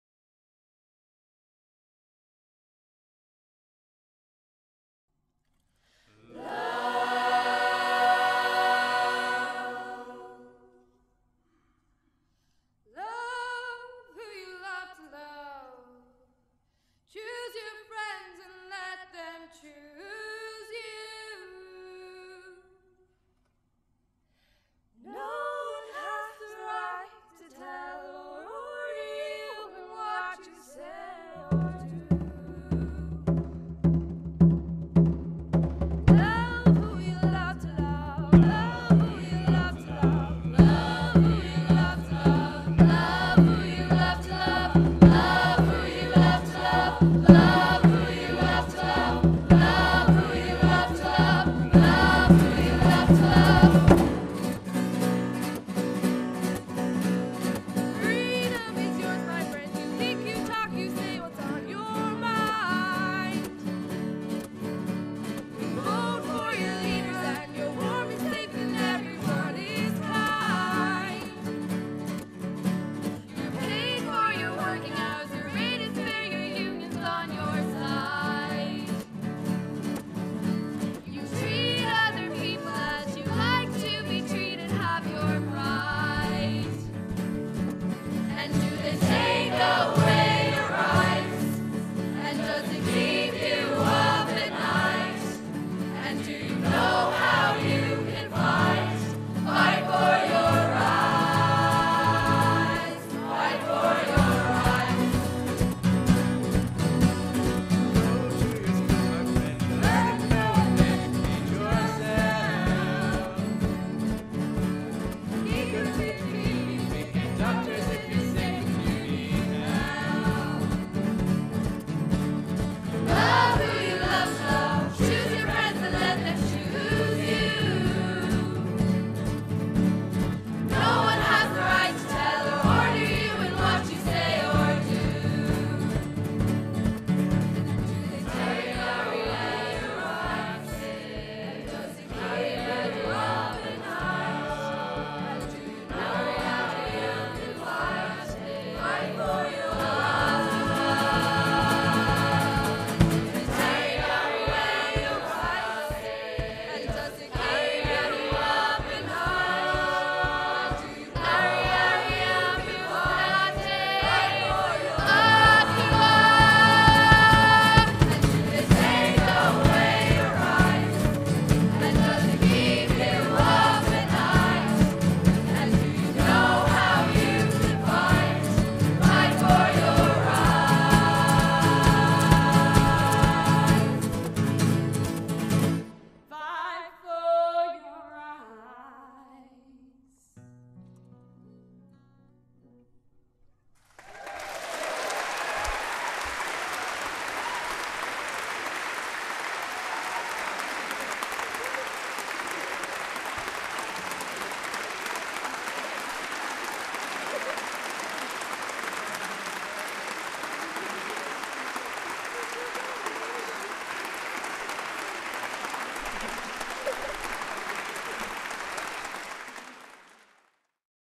Amnesty International's 50th Anniversary
Gort School Choir sing for Amnesty International's 50th Anniversary